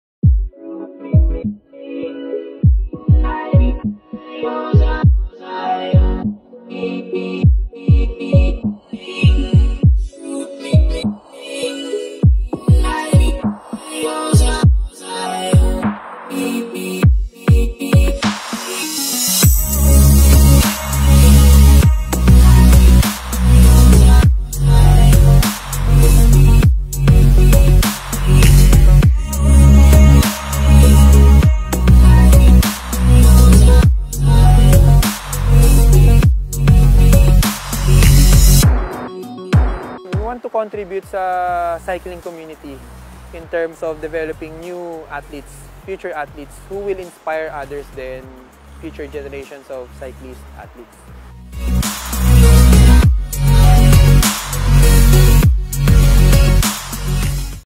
Hit The Trails Even if it Rains
Action Sports Hub is located at Eugenio Lopez Center Sumulong Highway, Antipolo Rizal. We're looking forward sa mga future developments nila to become an internationally recognized bike trail and venue for learning and developing skills ng athletes. According to Mike, yung trails naten dito needs to level up para pag dating ng filipino athletes sa ibang bansa, mas familiar and carry nila yung challenges ng trails doon...